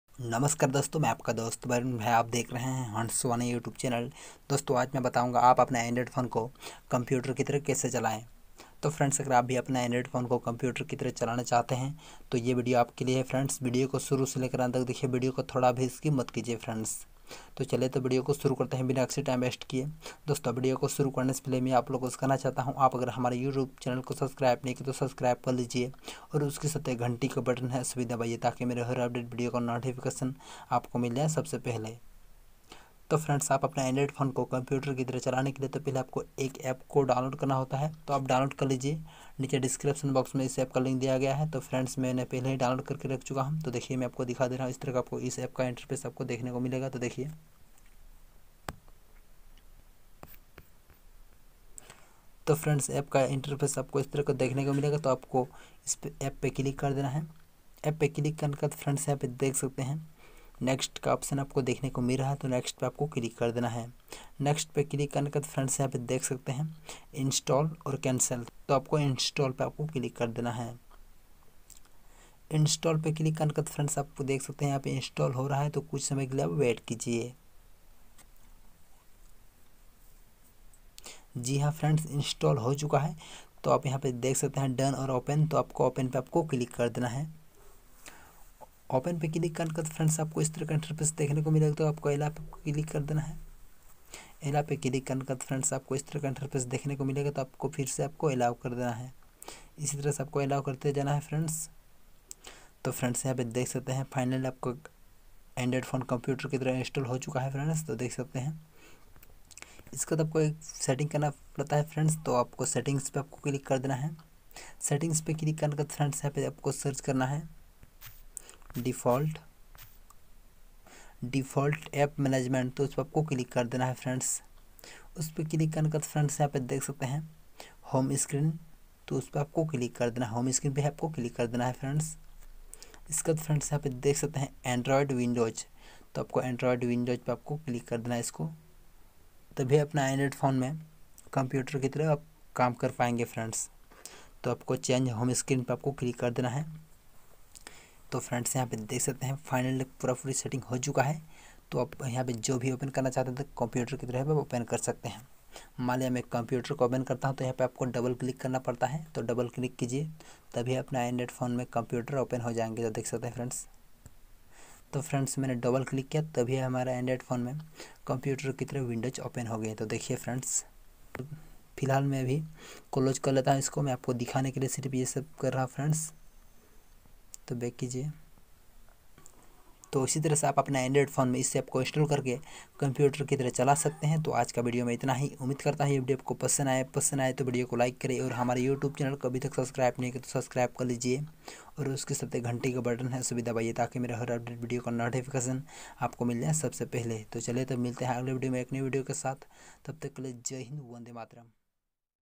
एंड्रॉयड फोन को कंप्यूटर की तरह कैसे चलाएं?|| How to run Android phone like a computer?
Namaskar Dosto,
                               Main aapka dost Barun Bhai. Aap dekh rhe hai. HANS  VANI YouTube channel.
Iss video me bataye hai. एंड्रॉयड फोन को कंप्यूटर की तरह कैसे चलाएं?|| How to run Android phone like a computer? Dosto is video ko suru se lekar End tak dekhe.